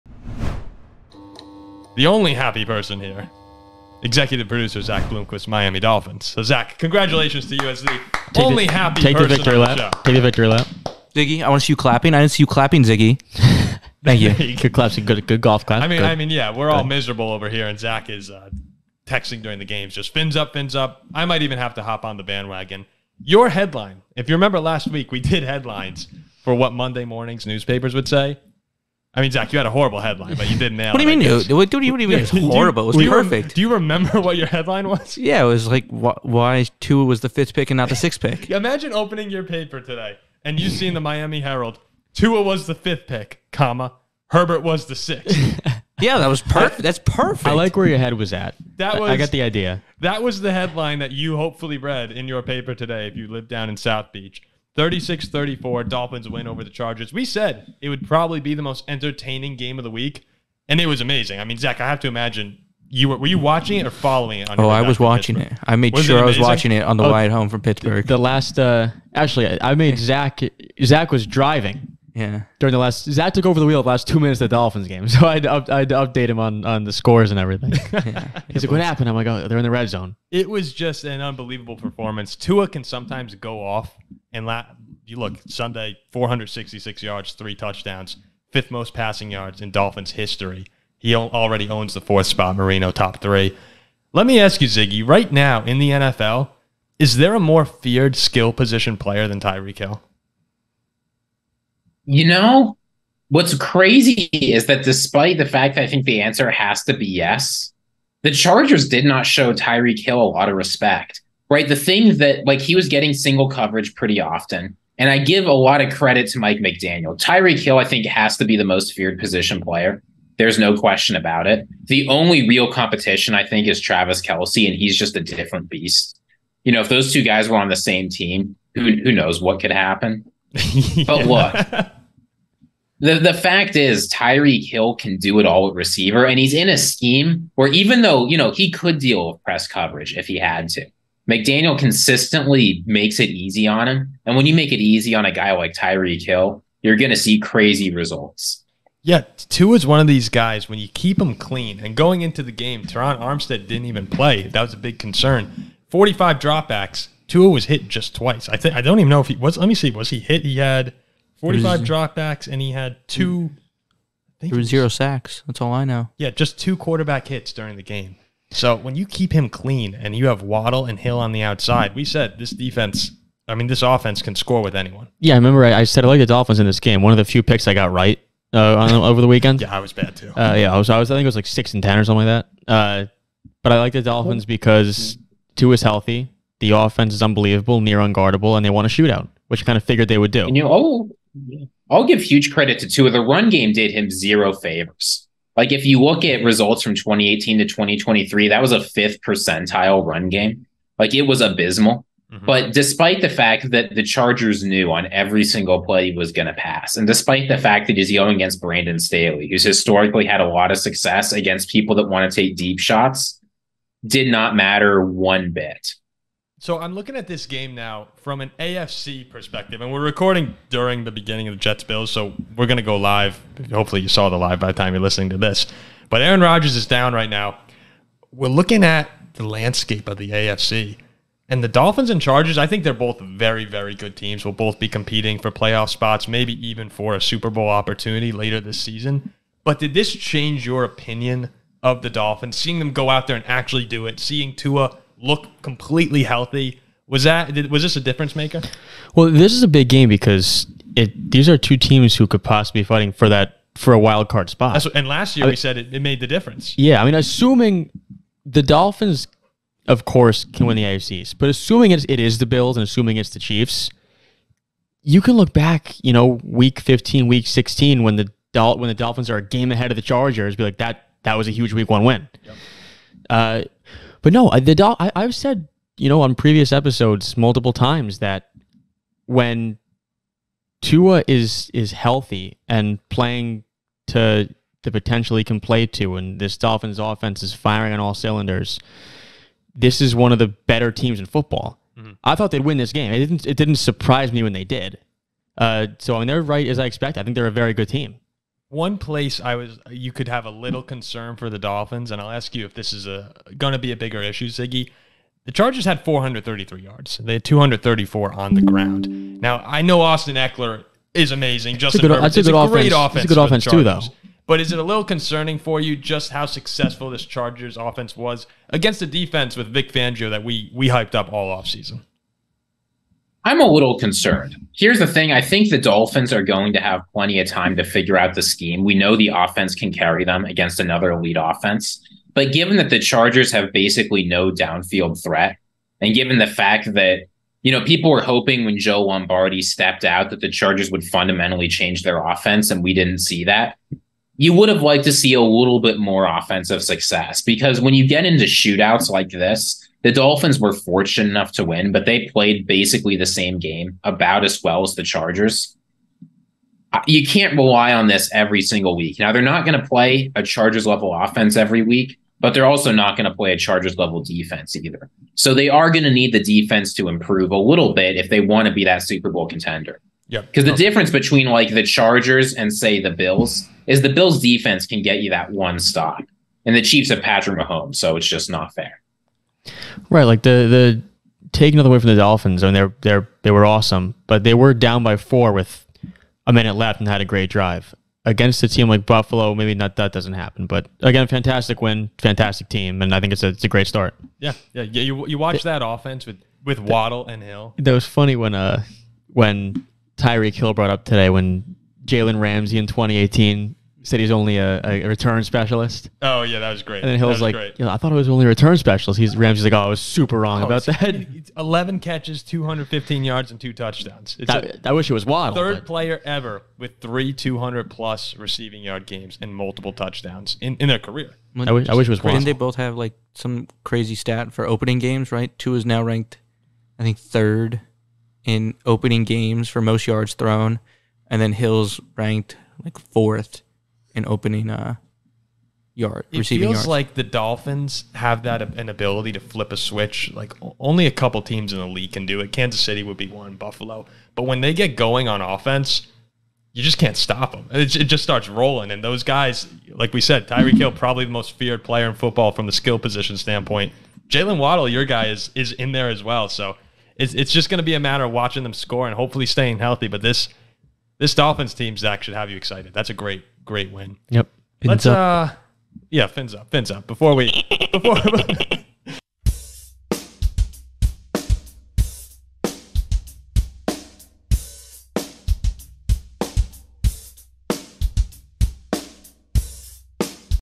Super Bowl is ON! Tua & Tyreek Hill EXPLODE in Dolphins win vs. Justin Herbert & Chargers | PFS
Paul Farrington, Jack Weinberger, and Robert "Ziggy" Ziegler react to Tua Tagovailoa, Tyreek Hill, and the Miami Dolphins' thrilling 36-34 win over Justin Herbert, Austin Ekeler, and the Los Angeles Chargers. The guys debate whether Tyreek Hill and Jaylen Waddle give the Dolphins the most dangerous offense in the NFL and whether they now view Miami as a more serious threat to represent the AFC in the Super Bowl.